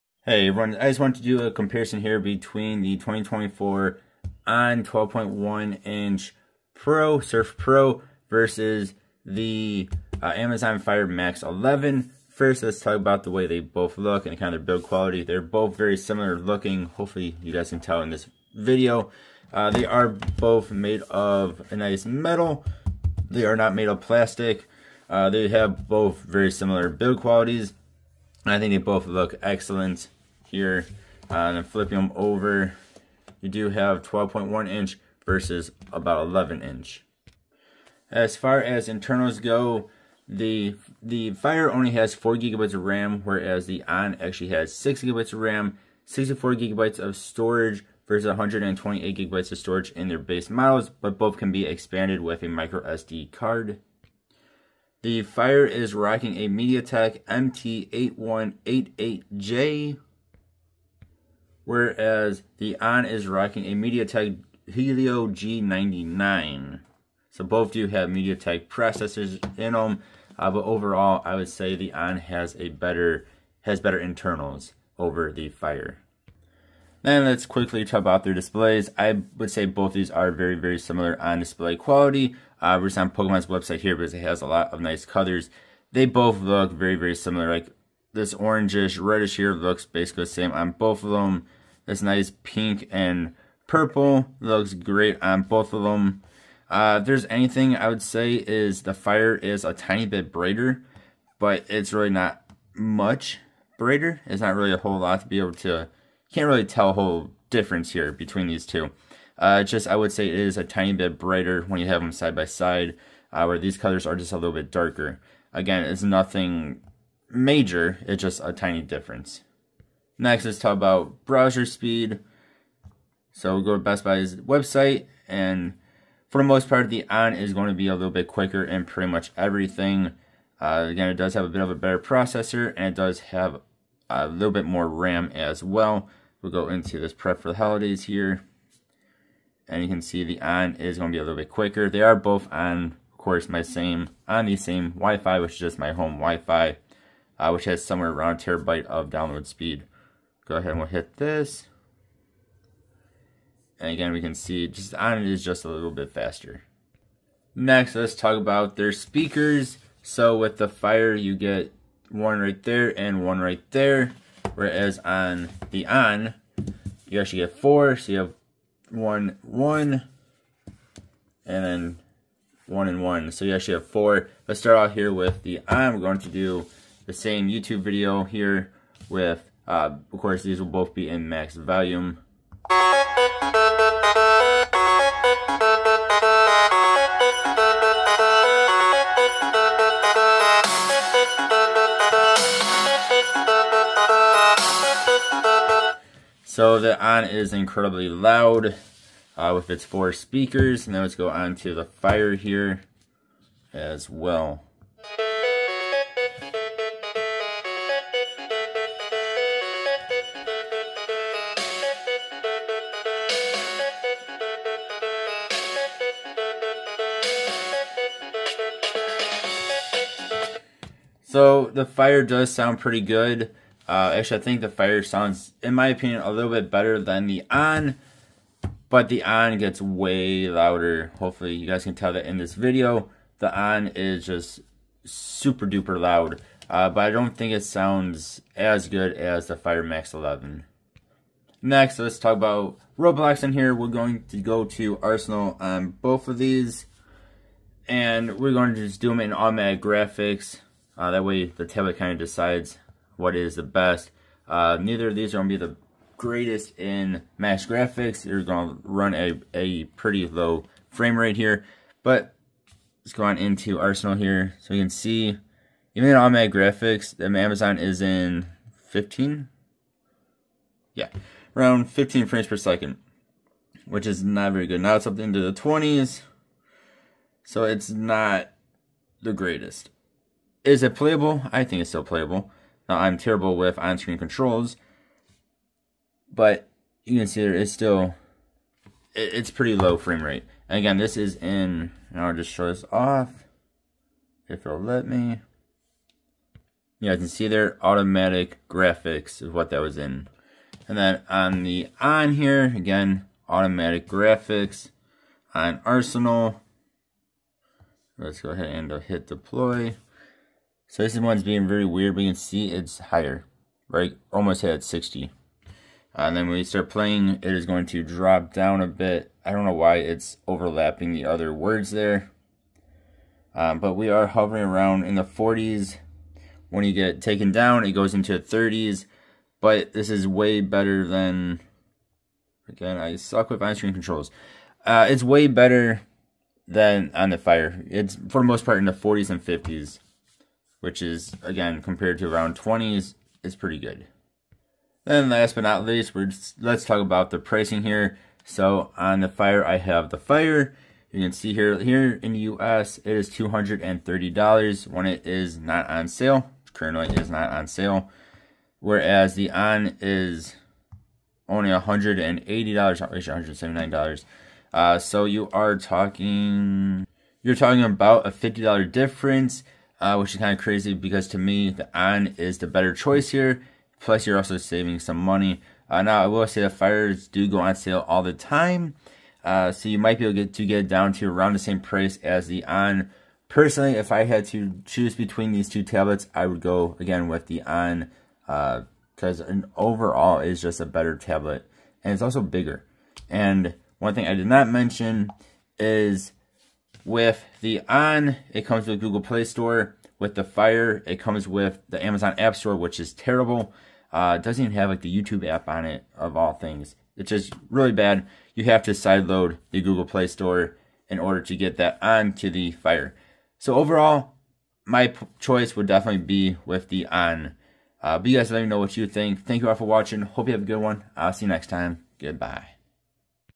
2024 Onn Surf 12.1 Vs Amazon Fire Max 11
My thoughts and opinions on the 2024 Onn Surf Pro 12.1 Vs Amazon Fire Max 11.